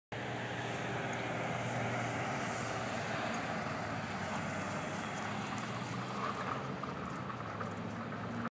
A dog is playing tennis ball
A black Lab  is playing  tennis ball